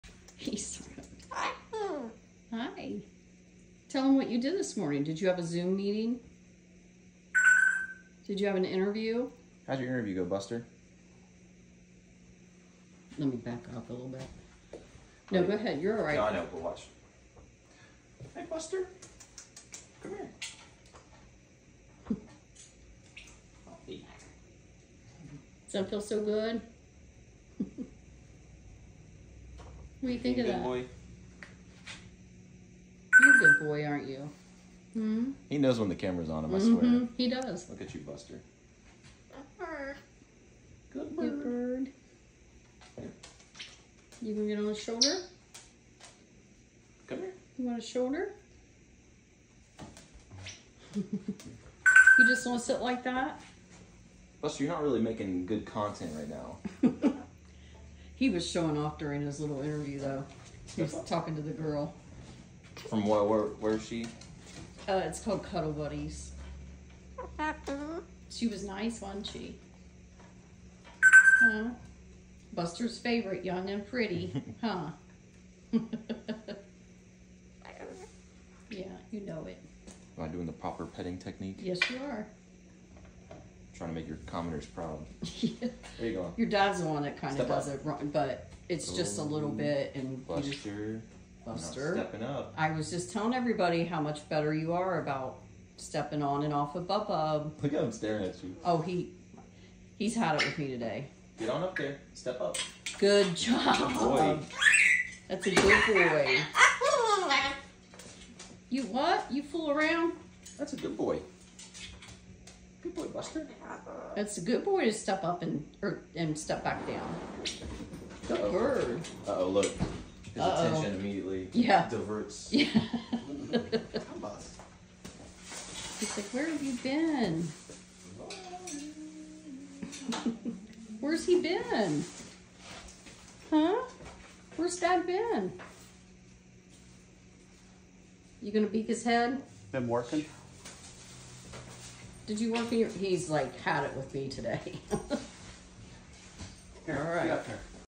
Buster: Hi Bub, I good bird
Buster always enjoys sibling time. He did have to be told twice to step off. He is doing so much better with Bub now. He has been working with him on this every time he visits